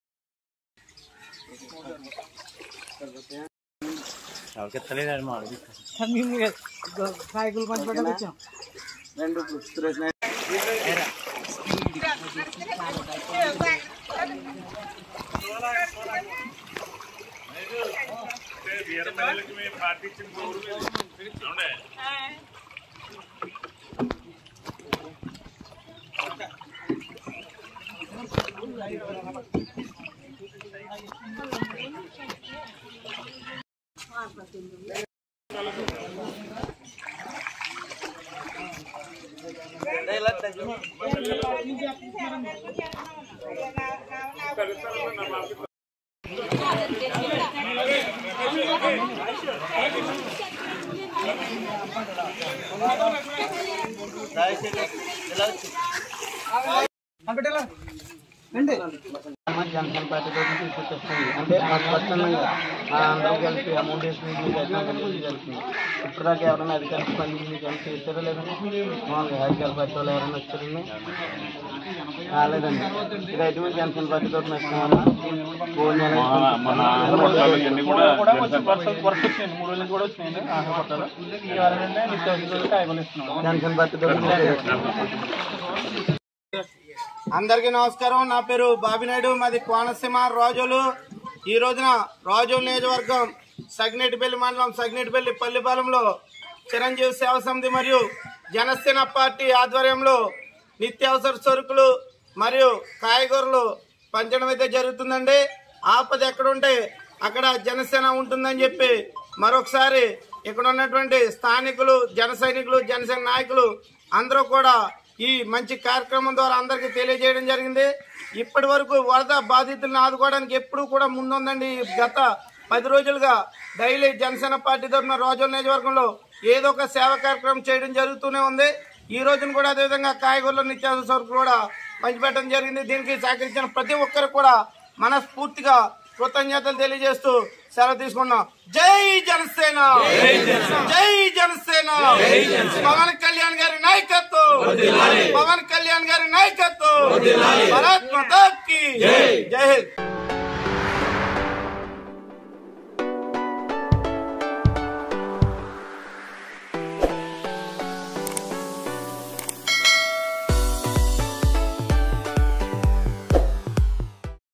రాజోలు వరద బాధితులకు జనసేన నిత్యవసర సరుకులు పంపిణీ | Razole Janasena Leaders Distributed Groceries
Konaseema Dist, Razole Janasena party leaders Bobby Naidu, and Janasena activists distributed Groceries and essential commodities to the Godavari flood victims in 2022.